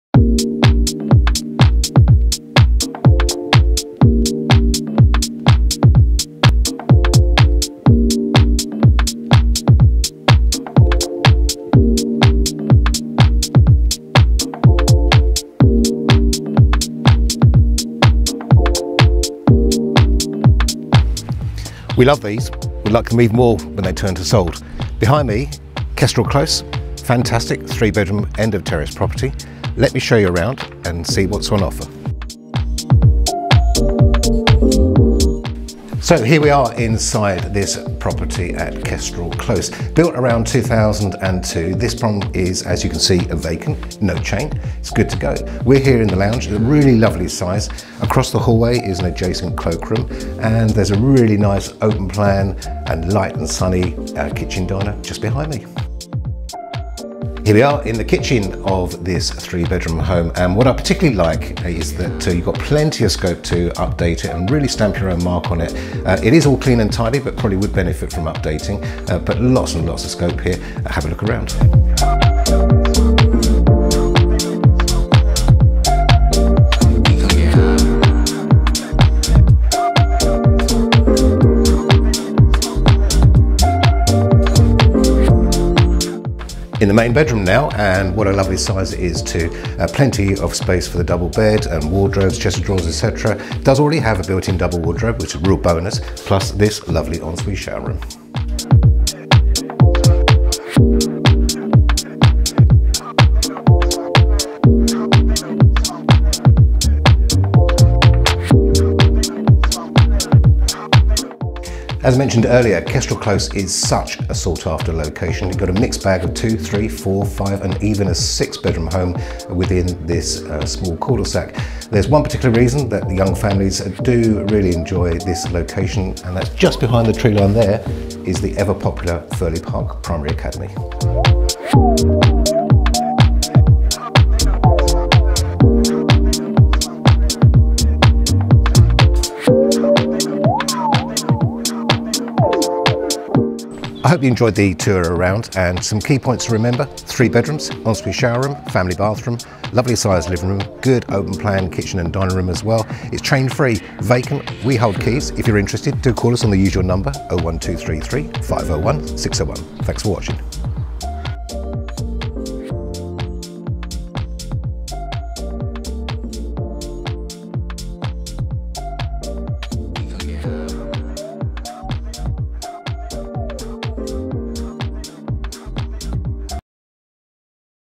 ID1814 - Kestrel Close, Park Farm, Ashford TN23 3RB
Situated on the sought after Park Farm development in Ashford is this spacious three bedroom home which is offered for sale chain free!

On the ground floor the front door opens to the hallway with adjacent cloakroom, stairs to the first floor and doors to the spacious lounge which has a window to the front and door to the dining room. The dining room has a door and window to the garden and opening to the kitchen where there are wall and base units, work surfaces, inset sink and space for appliances.

On the first floor there are three bedrooms and the family bathroom. The main bedroom has built in wardrobes, window to the front and door to the en suite shower room which has a tiled shower cubicle, w.c and wash hand basin. Bedrooms two and three overlook the rear and the bathroom has a white suite with panelled bath, w.c and wash hand basin.

Outside, the garden is enclosed and laid to lawn with side pedestrian access. To the front there are parking spaces although not allocated to the property.